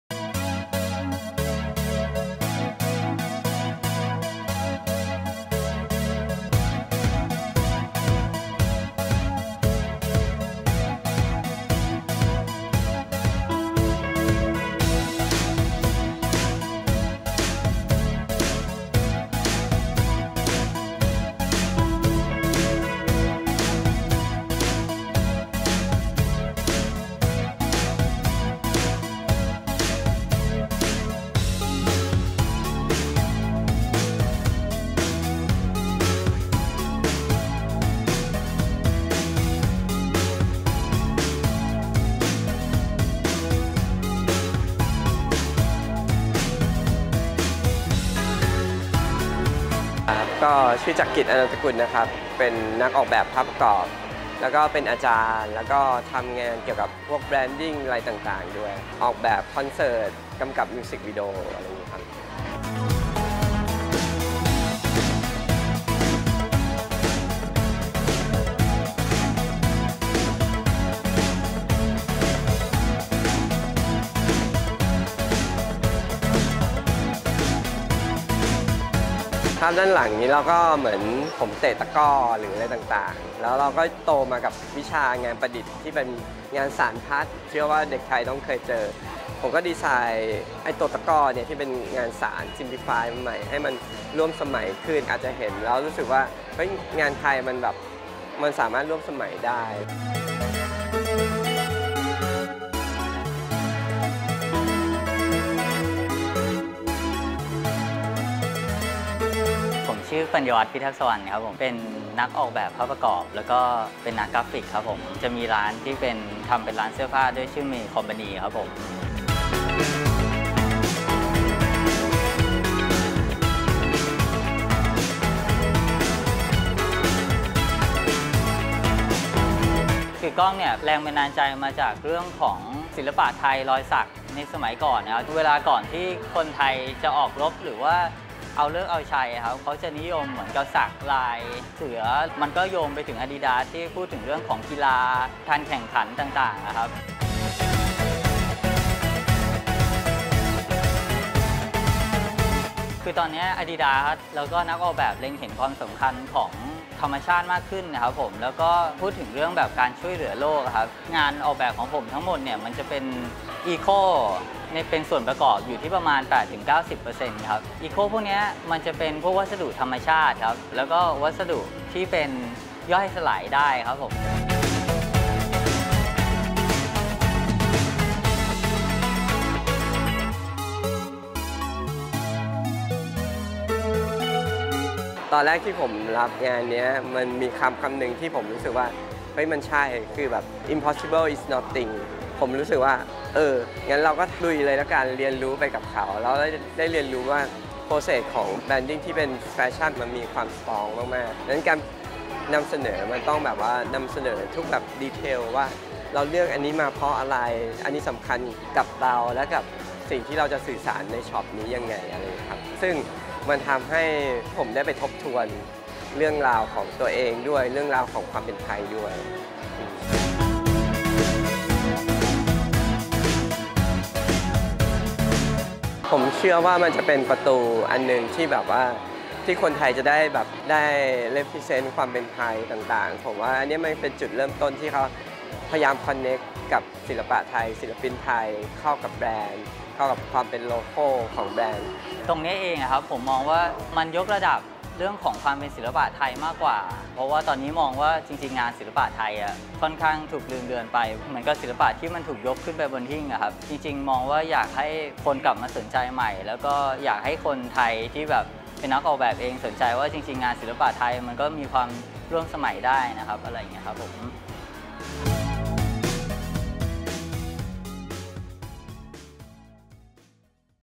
เปิดแนวคิด "แบรนด์ เซ็นเตอร์" ใหม่ของ Adidas สาขาแรกที่ตกแต่งด้วยฝีมือศิลปินไทย
เบื้องหลัง "แบรนด์ เซ็นเตอร์" ใหม่ของ Adidas ในไทยที่สยามสแควร์วัน สาขานี้เป็นสาขาแรกที่ออกแบบการตกแต่งโดยศิลปินไทย ตามนโยบายการ 'localize' ปรับแบรนด์ให้เข้ากับท้องถิ่น

ไปชมแนวคิดของศิลปินไทยทั้ง 2 คน คือ “ปัญญวัฒน์ พิทักษวรรณ” หรือ Tos Panyawat และ “จักรกฤษณ์ อนันตกุล” หรือ Hello iam JK กัน!